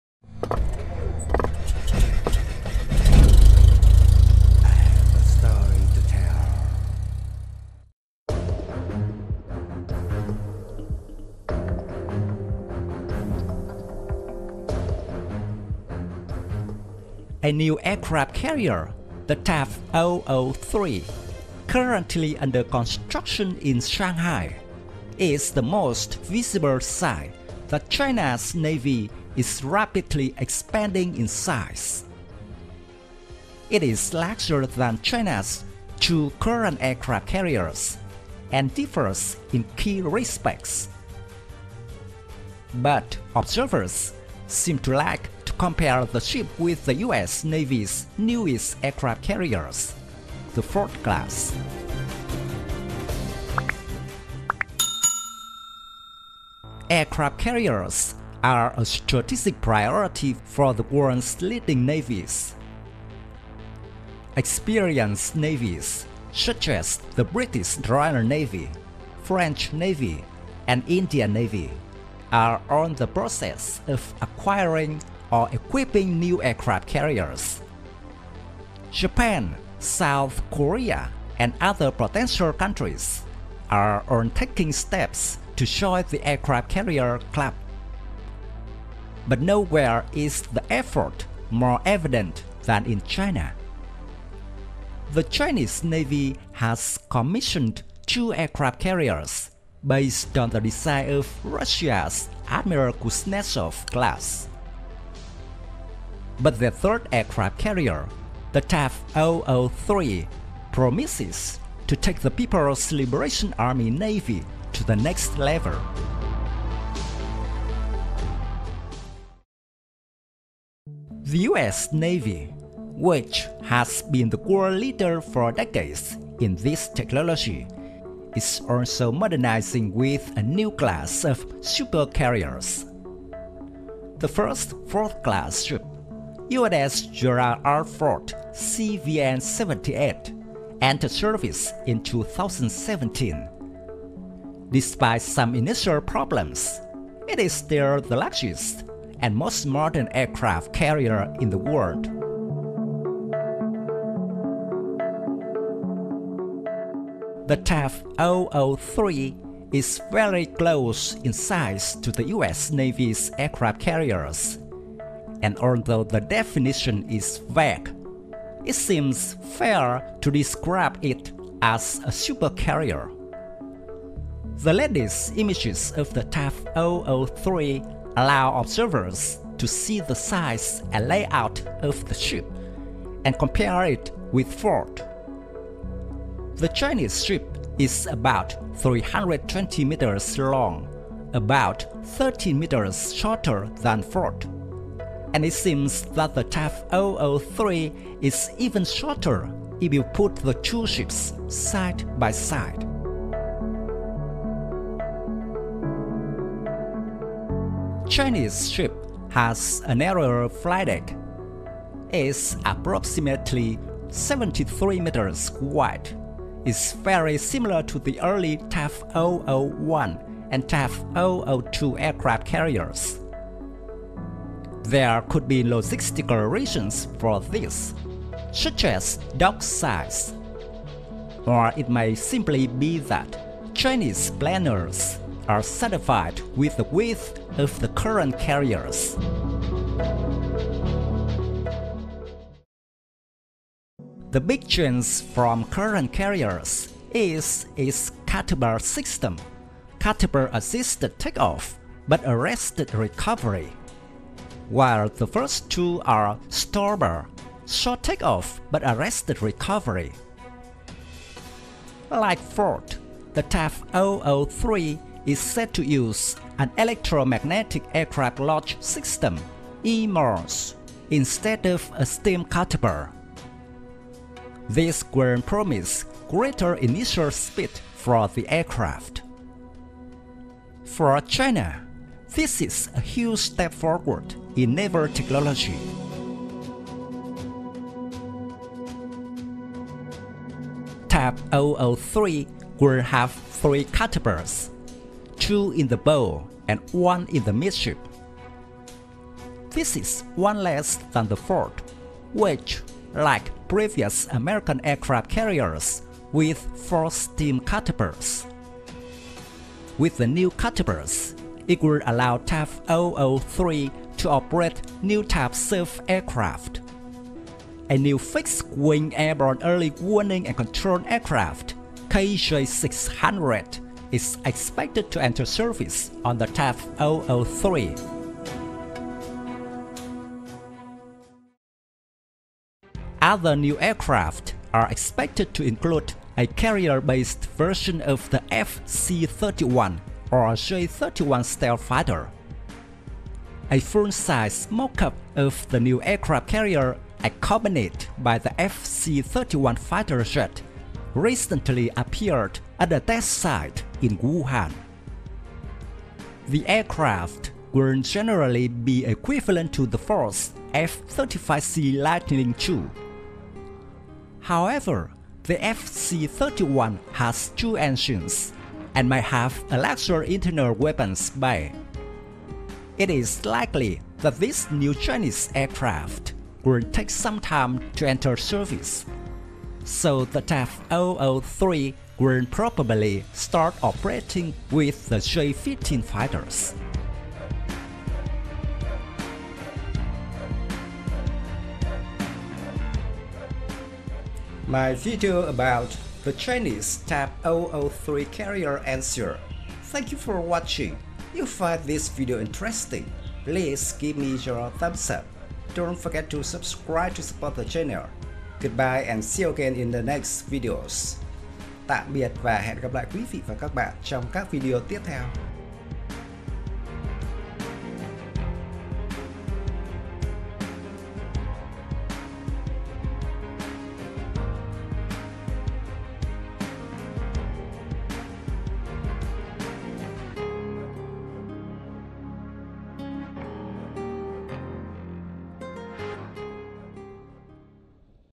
China's Type 003 compared to the US super carrier?
A new aircraft carrier, the Type 003, currently under construction in Shanghai, is the most visible sign that China's navy is rapidly expanding in size. It is larger than China's two current aircraft carriers and differs in key respects. But observers seem to like to compare the ship with the US Navy's newest aircraft carriers, the Ford-class.

Thank you very much.
Best regards
Dung Tran.